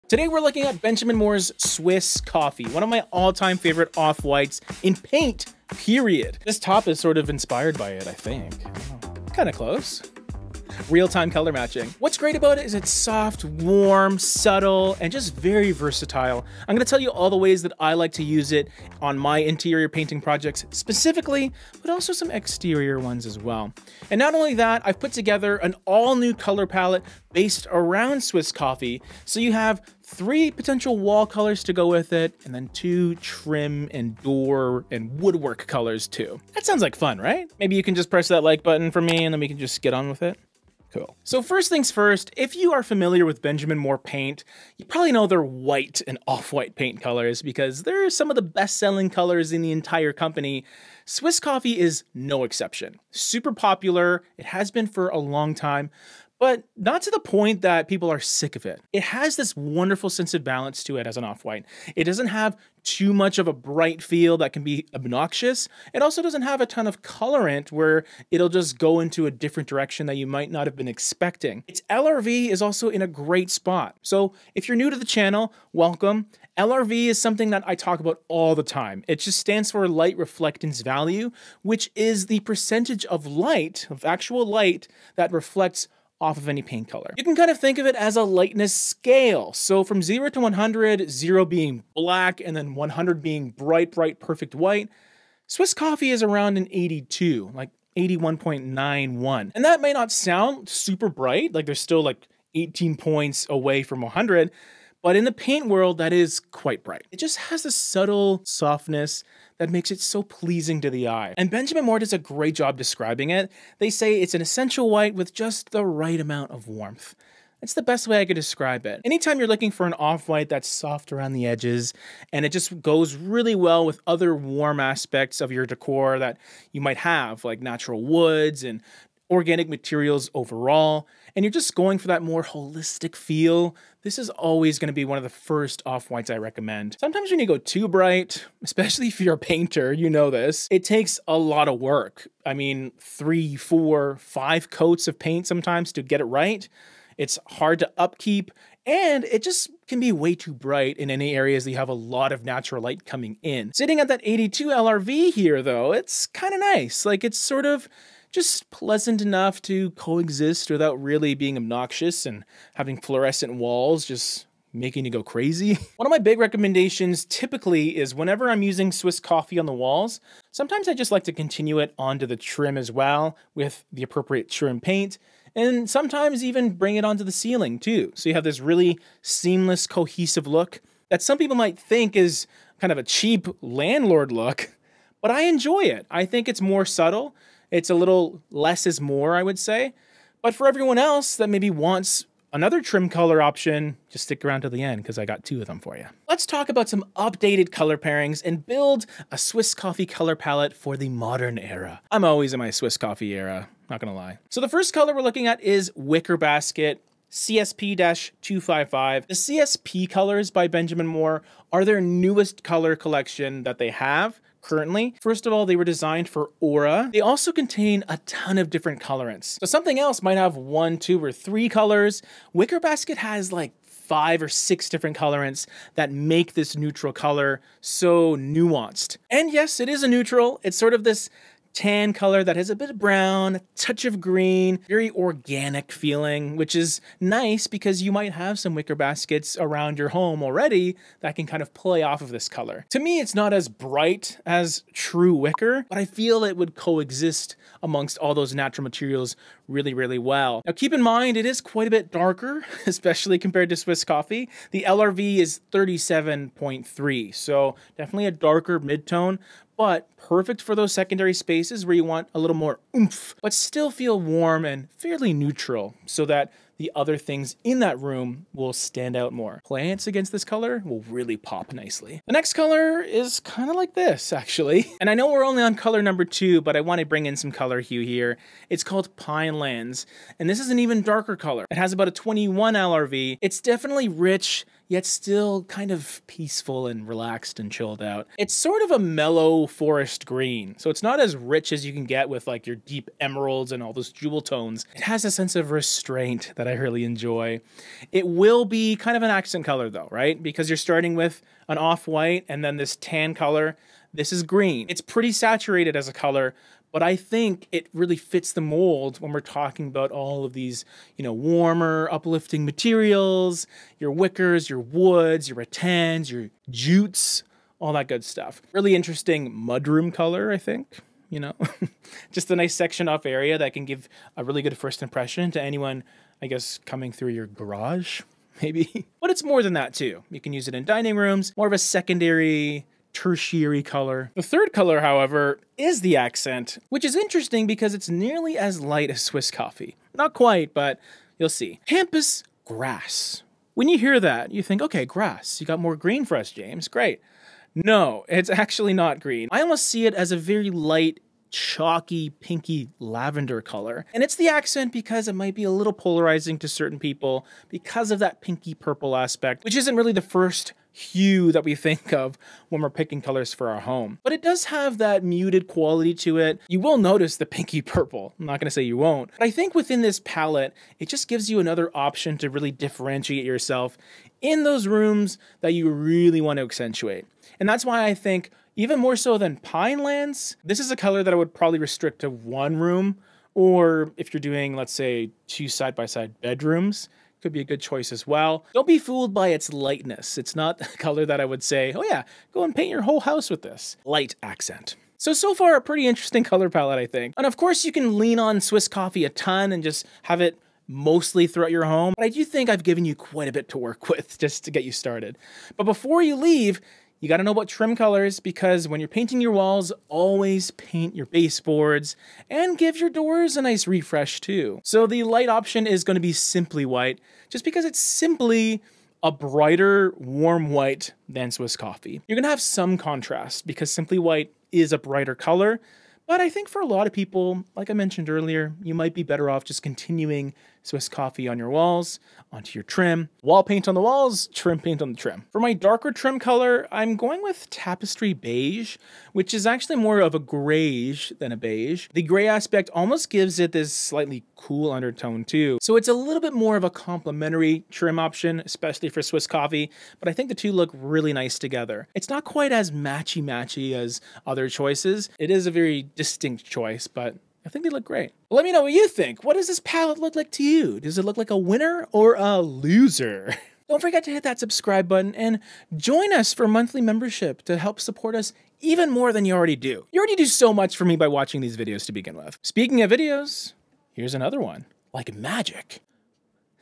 Everyone is Going Crazy for Benjamin Moore "Swiss Coffee"... Here's Why
Custom Color Palettes In this video, we're going to review Swiss Coffee OC-45. This gorgeous color is everywhere and we wanted to know why people are going crazy for it!

If you're looking for a bright color to add to your home, then you need to check out Benjamin Moore's "Swiss Coffee"! This color is perfect for any room in your house, and it blends seamlessly with any other color you might have. Plus, we got some color pairings that you can use as well!